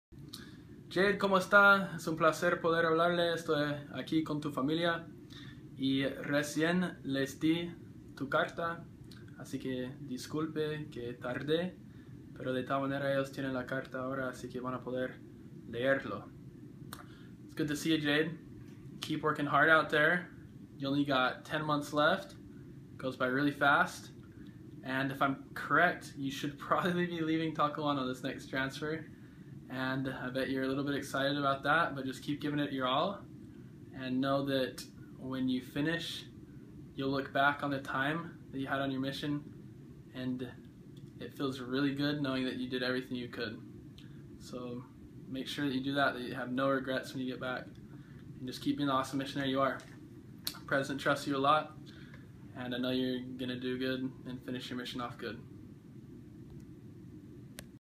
Jason Mitton's message to Elder Jade Smoot
Jason came by and visited before he left to go to school at SUU. He gave us the letter and I recorded this message while he was here.